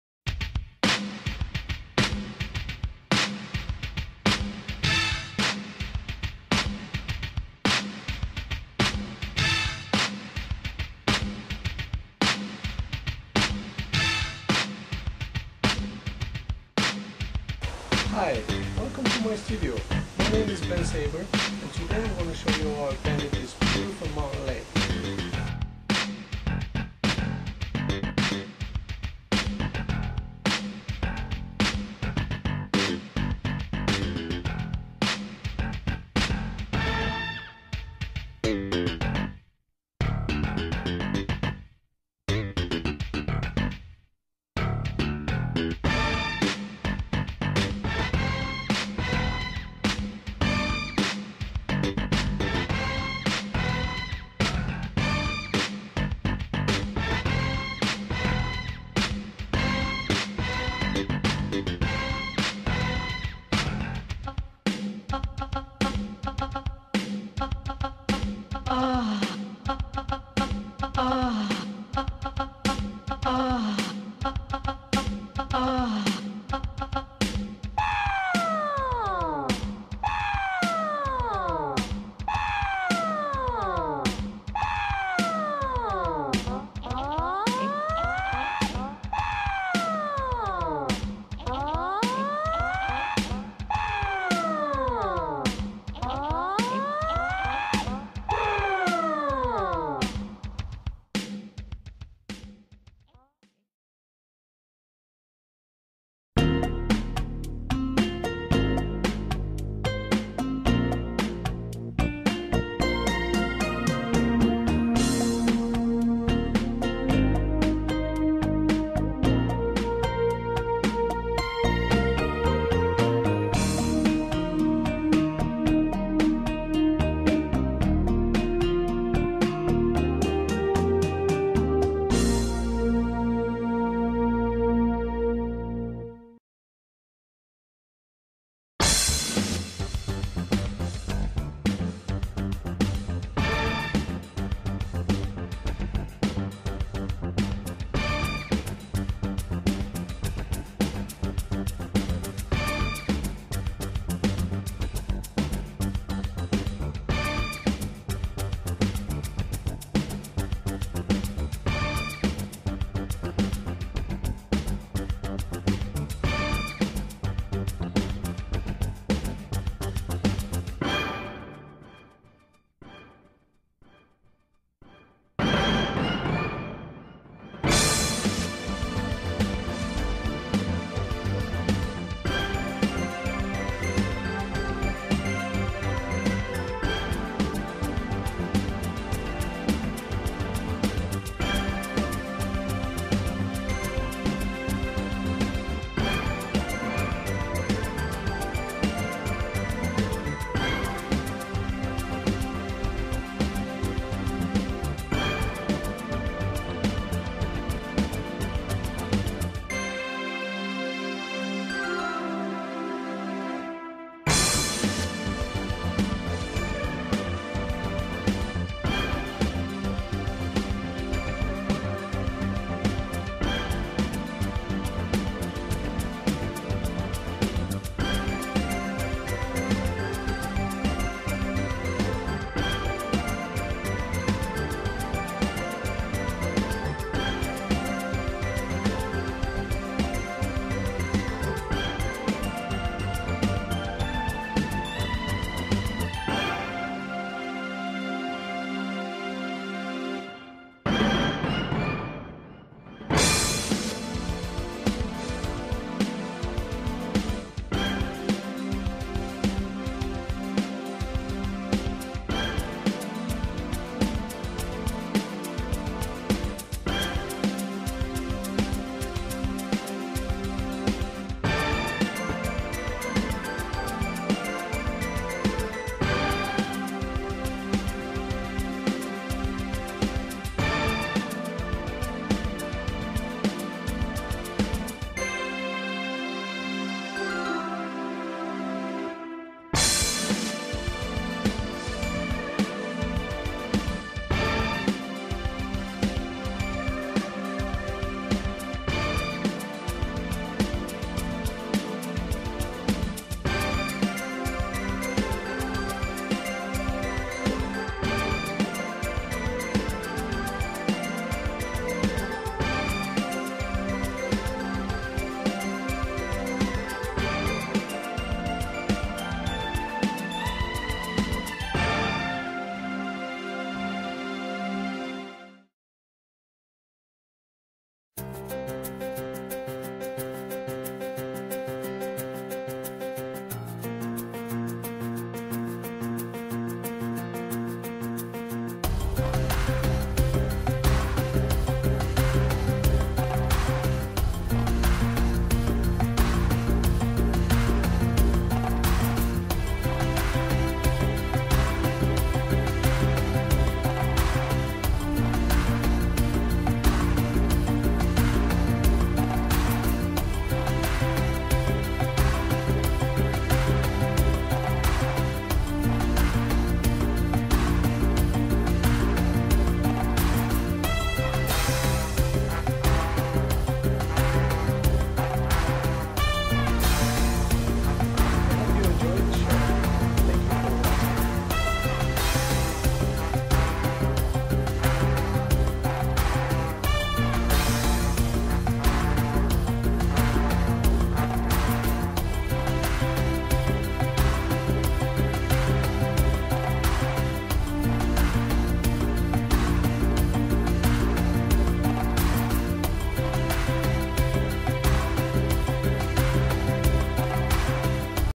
How To Paint Snow Mountain And Alpine Lake Fast Video Time Lapse
How to paint a snow mountain, alpine lake and evergreen trees using Liquitex acrylics on canvas 18x24 inches by artist/painter Ben Saber. If you like to watch a narrated slower video see my other youtube channel